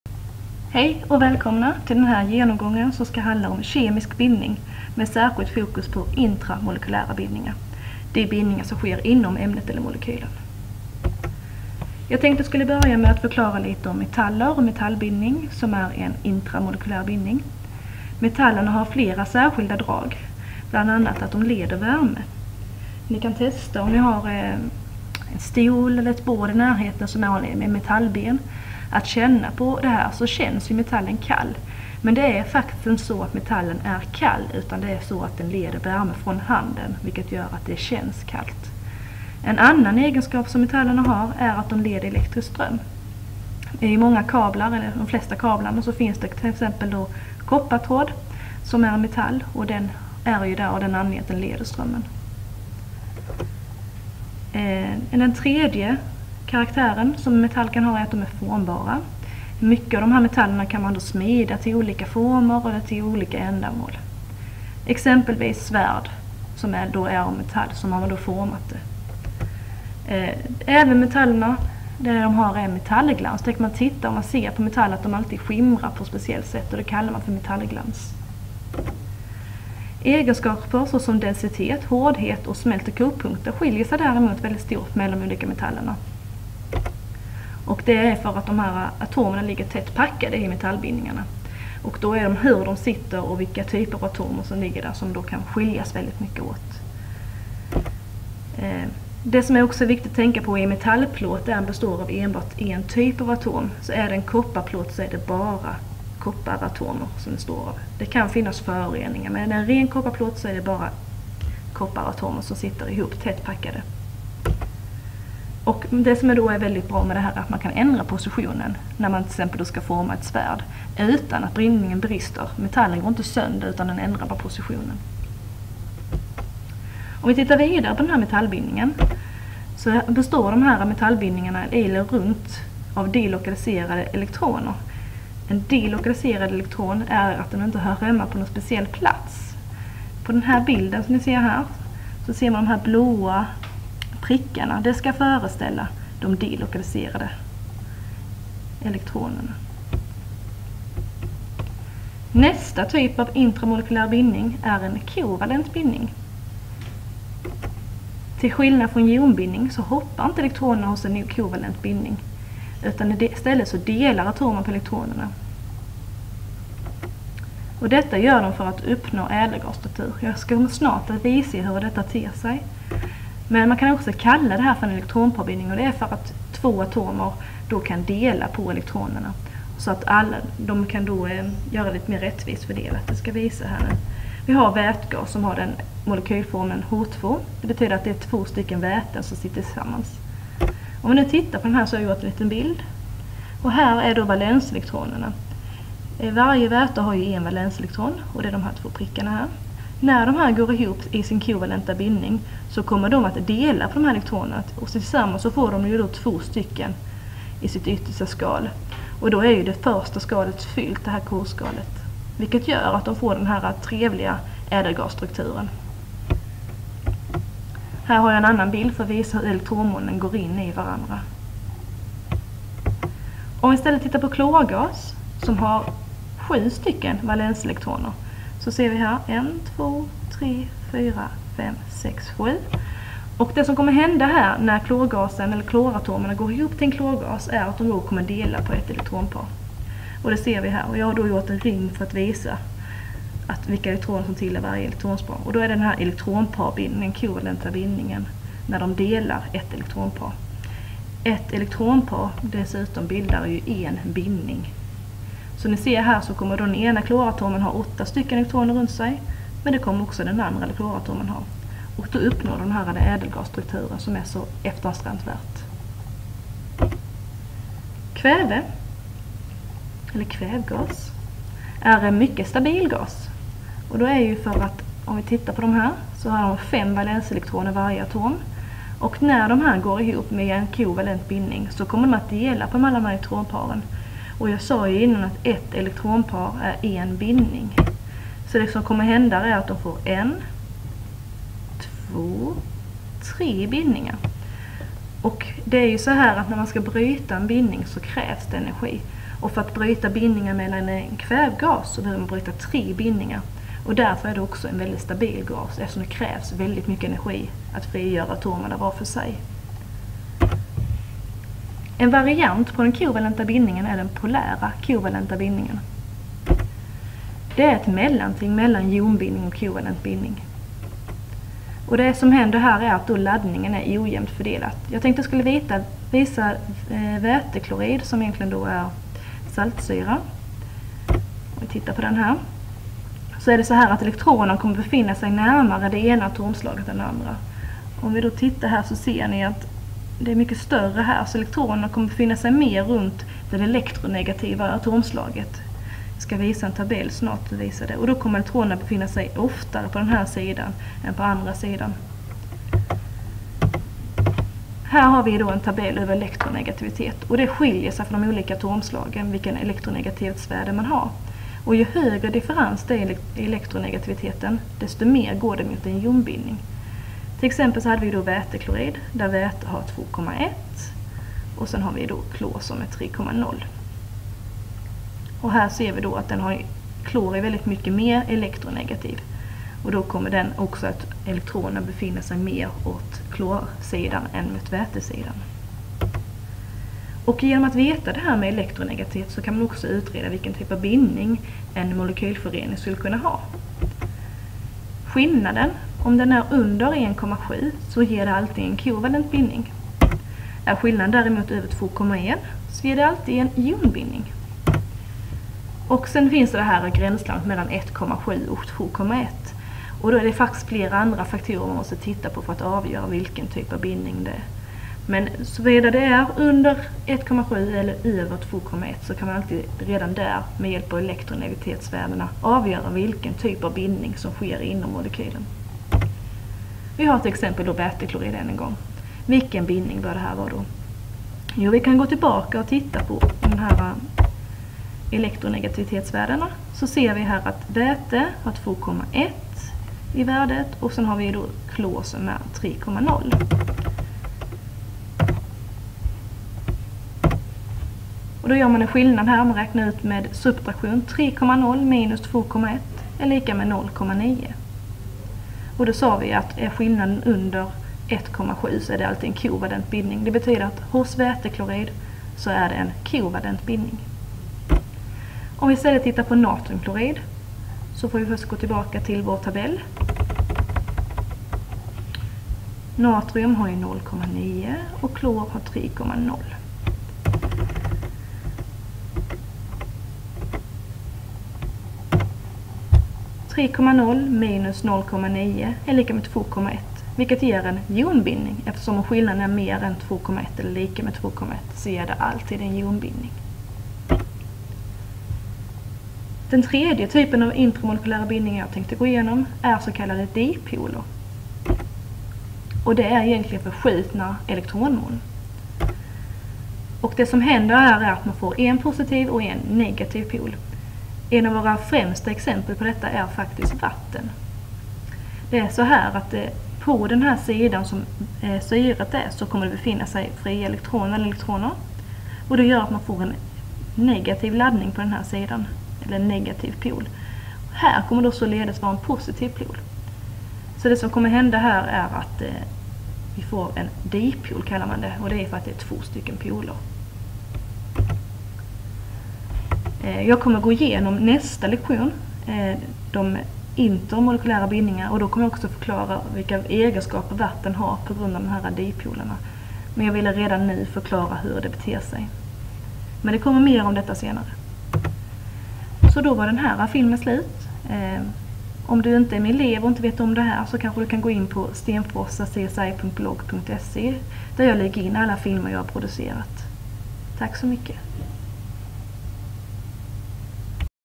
Intramolekylära bindningar
Filmer beskriver vilka olika typer av intramolekylära bindningar det finns. Används i flippa klassummet-lektion